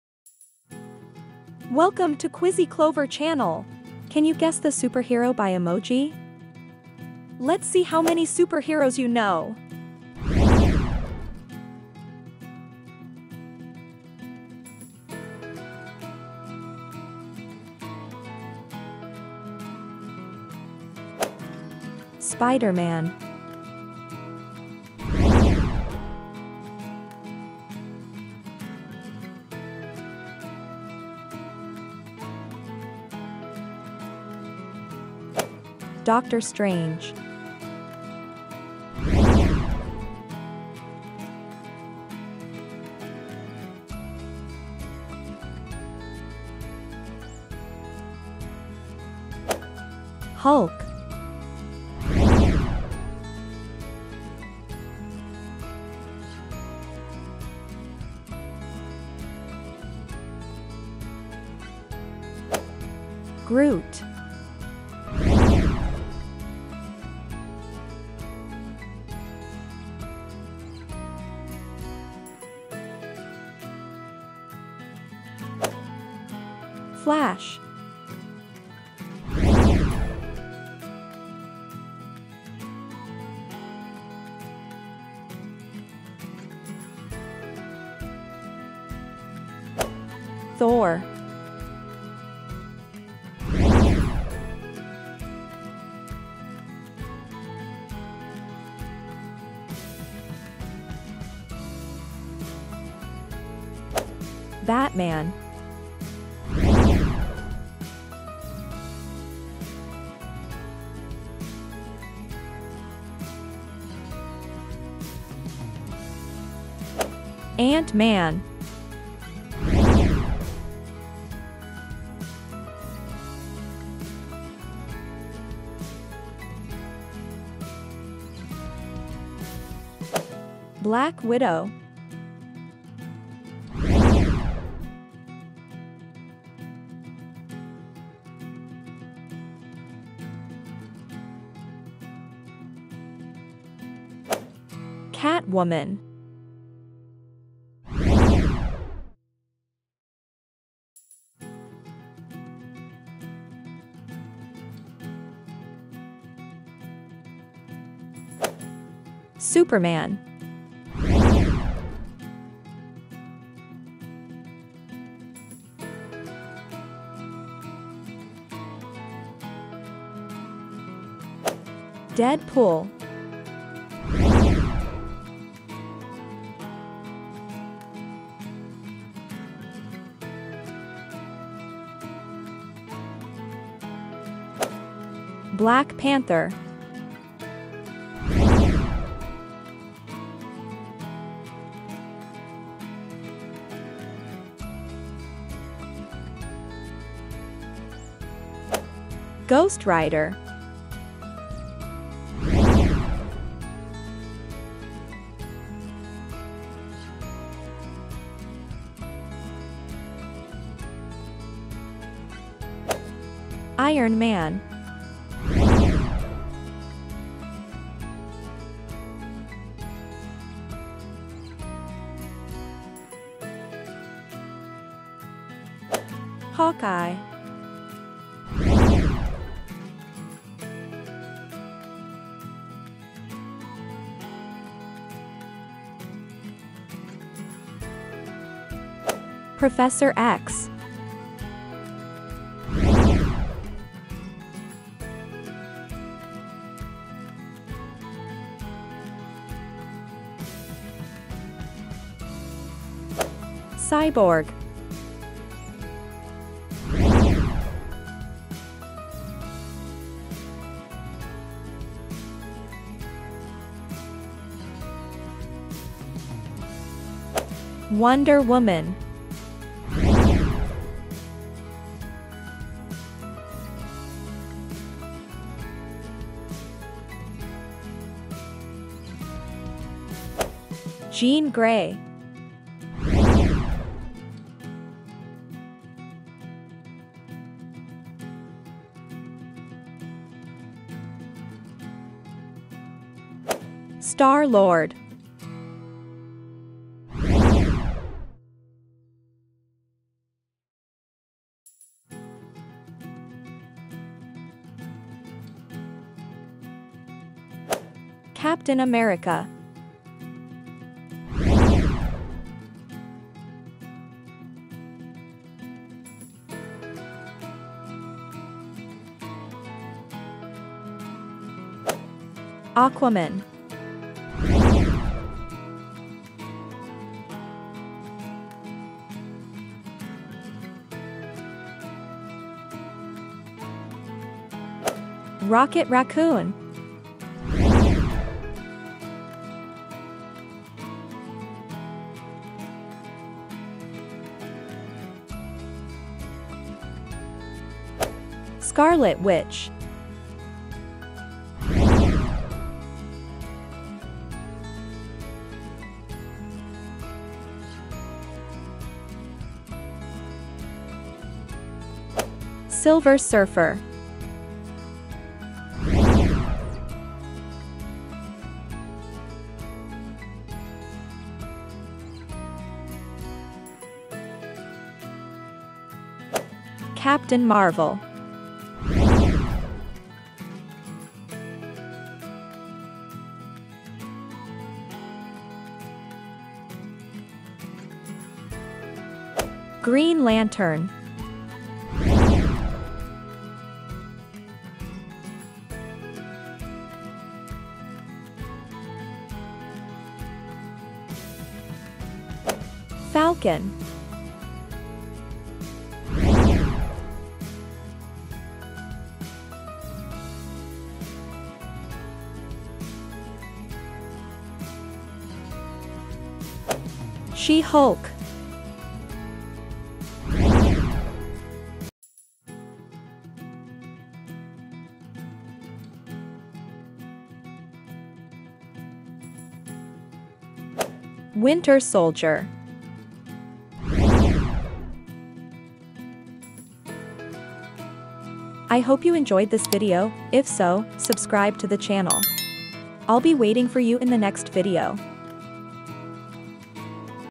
Guess The Superhero by Emoji! Superhero Quiz
Welcome to the Quizy Clover channel!
In this video you have to guess the superhero.all the superheroes in this video were taken from movies.I hope you've watched a lot of superhero movies😉
Good Luck and Write in The Comments How Much You Can Guess!